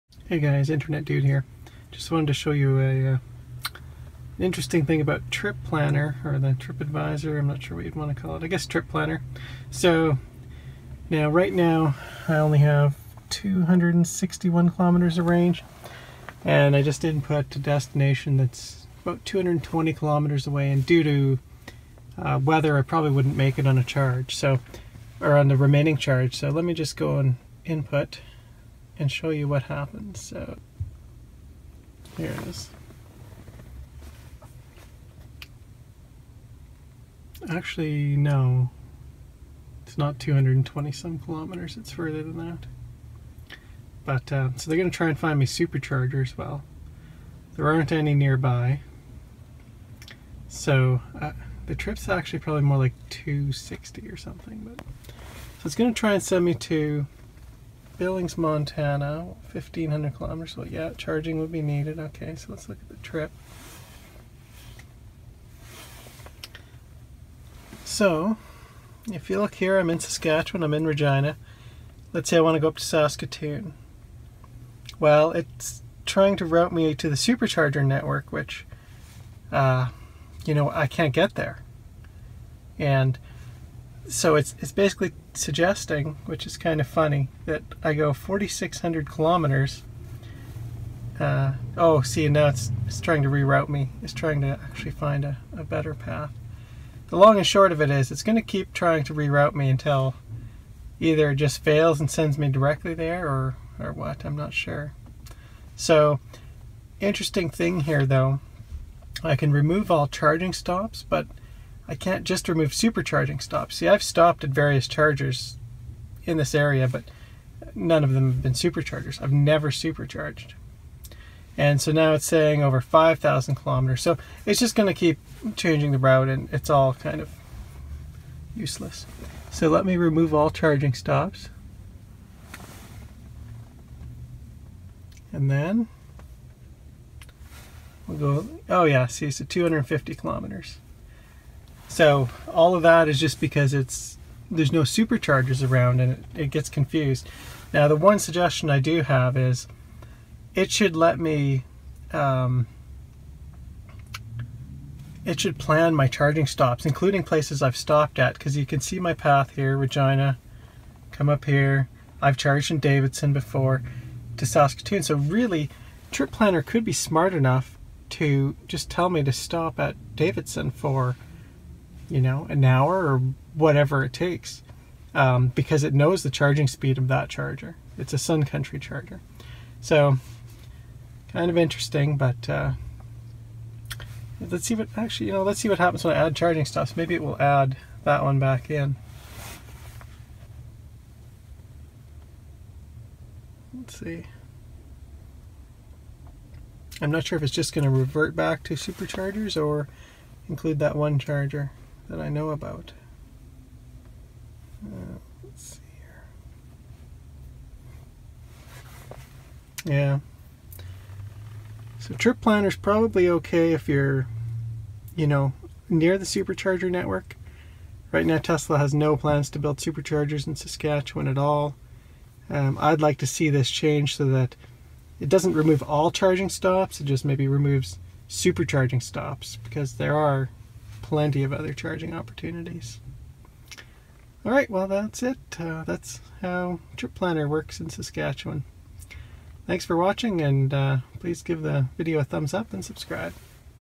Tesla Trip Planner With No Superchargers
When you have no superchargers anywhere even close, this is what Trip Planner does.

Suggestion for Tesla: Make trip planner smarter, let it use known, visited chargers to determine charge time needed for planned trip.